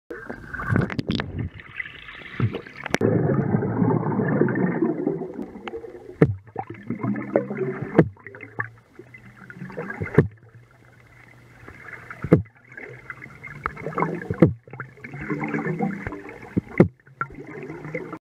Torpedo At The Pool
Playing with my underwater throwing torpedo.